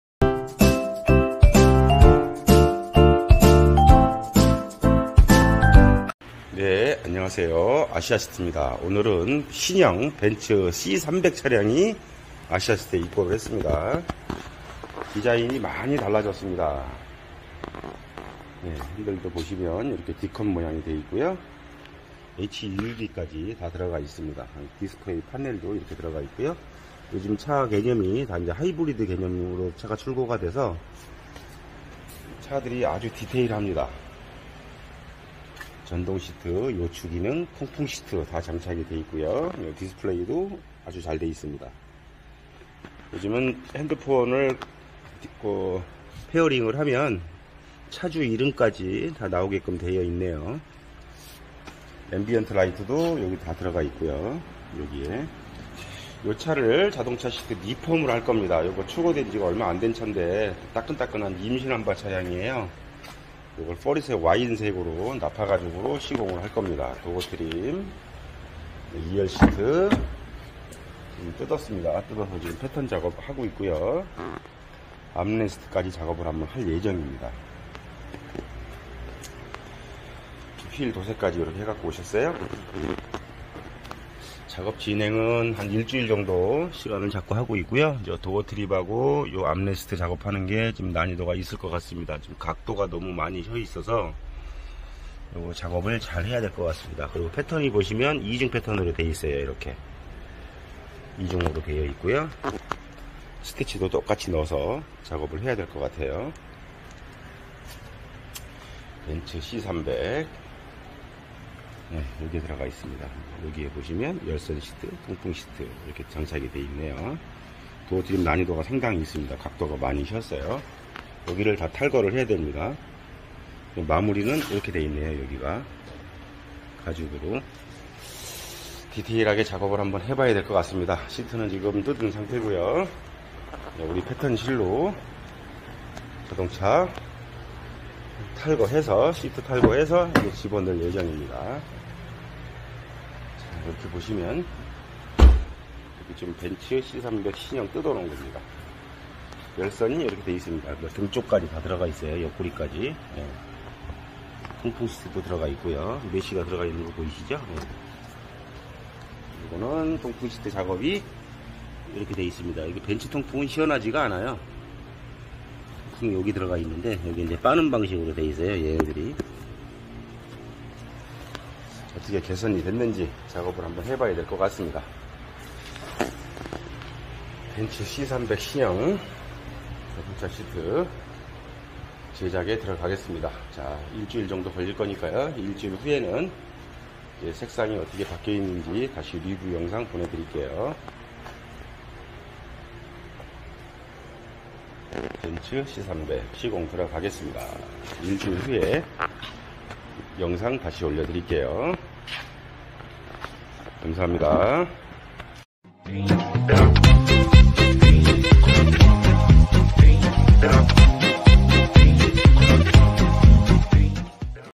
C300 신형 벤츠 아시아시트에서 신차출고후 자동차시트 커스텀 제작과정입니다 자동차시트 명가 아시아시트 문의주세요
벤츠c300신형 일산 아시아시트에서 커스텀시트 제작과정입니다 이쁘게 제작해드릴게요
이쁘게 제작해드릴게요 시공문의 일산동구 장항로114ㅡ아시아시트 010ㅡ8944ㅡ4985입니다